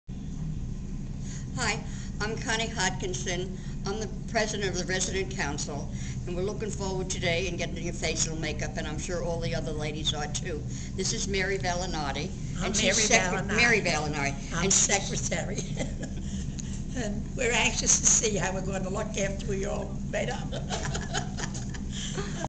Meet Connie and Mary!
Meet Connie and Mary of Nesconset. They share their story and how GlamourGals has impacted them.